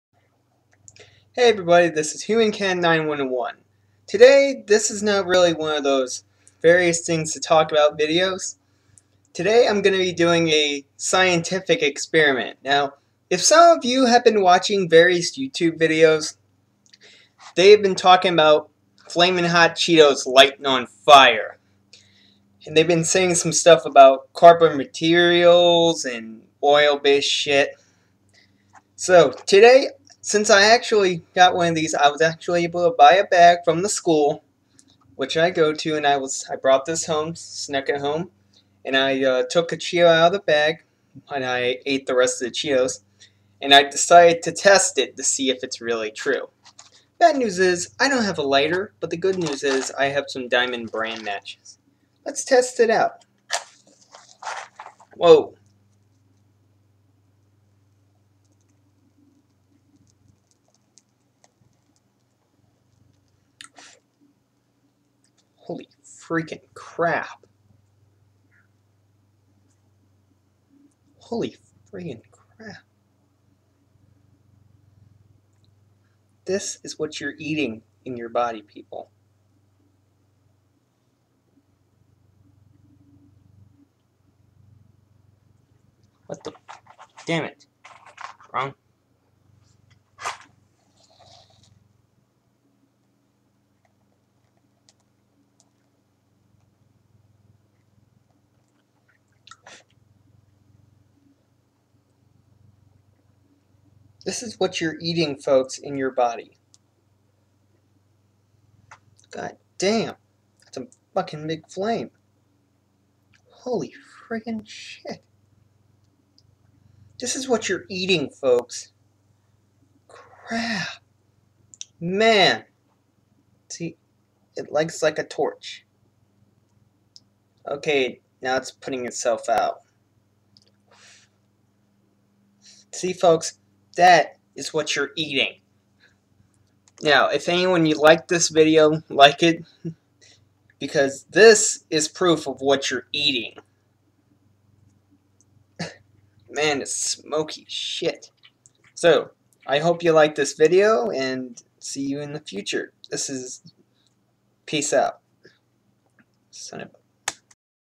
Science Experiment-Flamin' Hot Cheeto
This was a science experiment of the burning Flaming Hot Cheetos that I saw in various Youtube videos. DO NOT TRY THIS AT HOME!